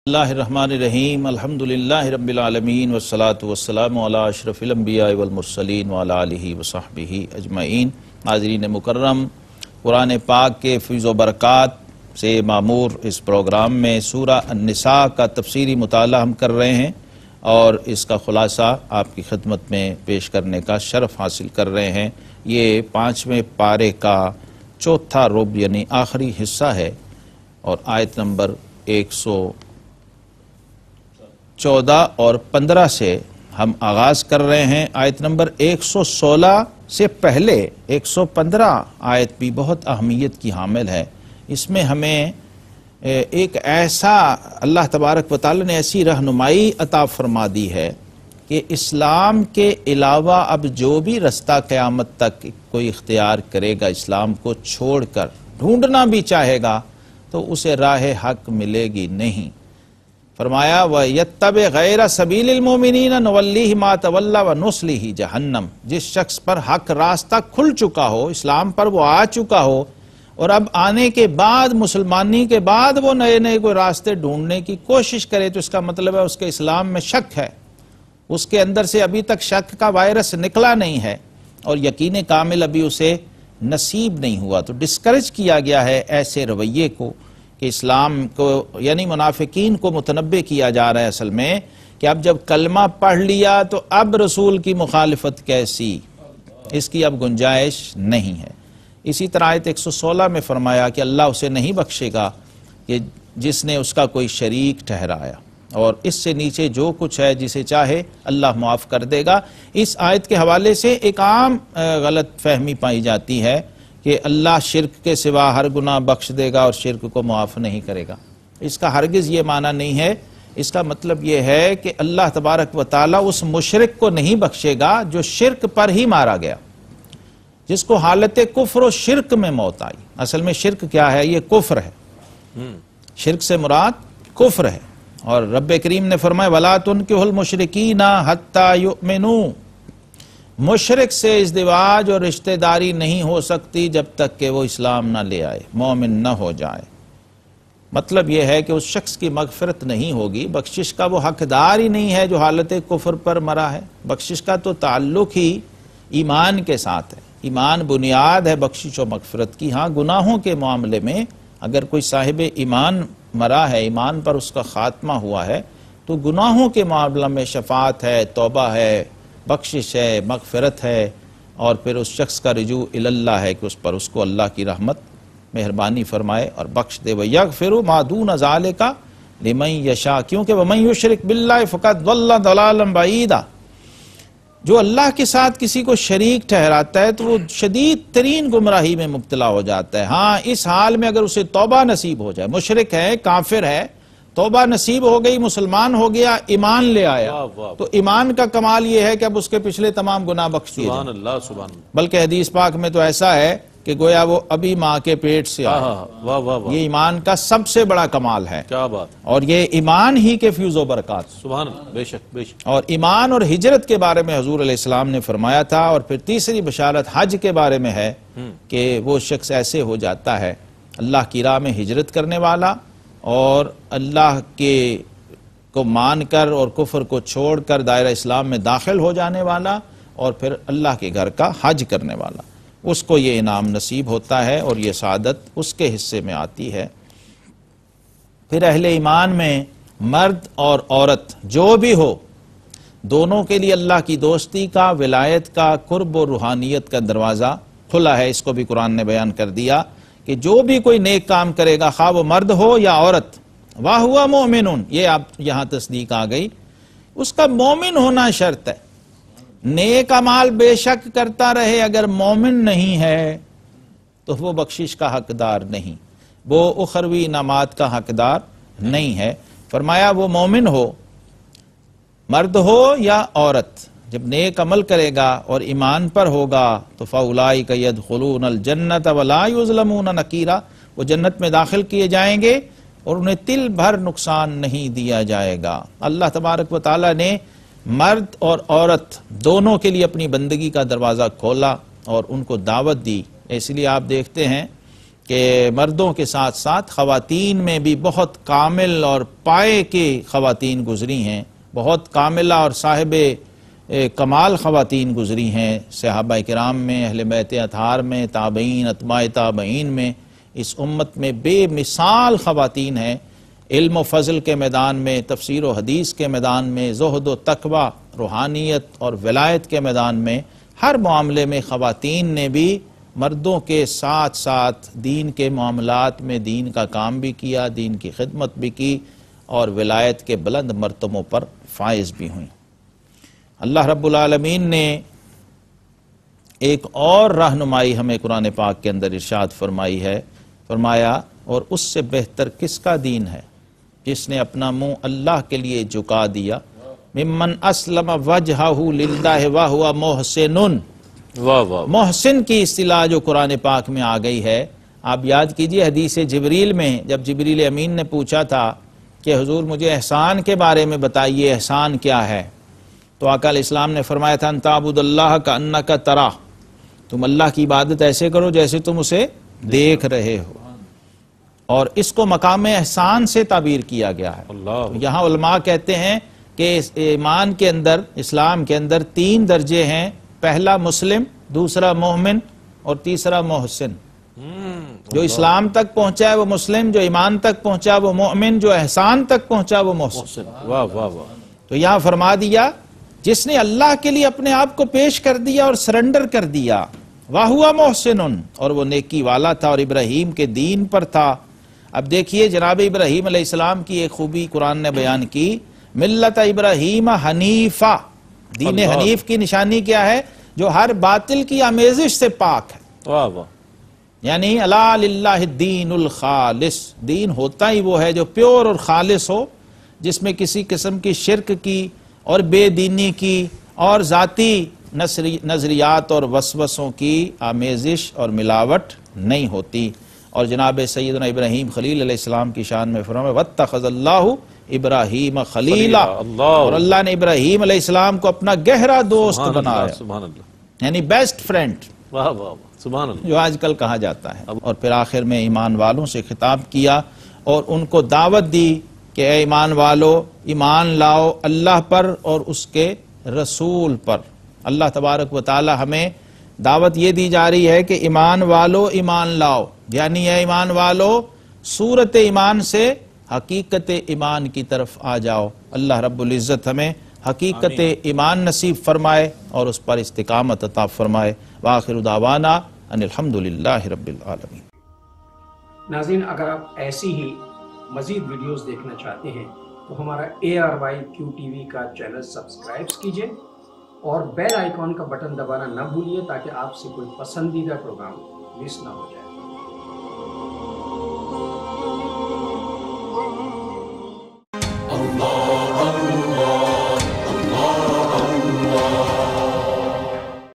Surah An-Nisa Ayat 114 to 147 || Qurani Ayat Ki Tafseer Aur Tafseeli Bayan
Paigham e Quran | Allama Shahzad Mujaddidi | Qurani Ayat Ki Tafseer Aur Tafseeli Bayan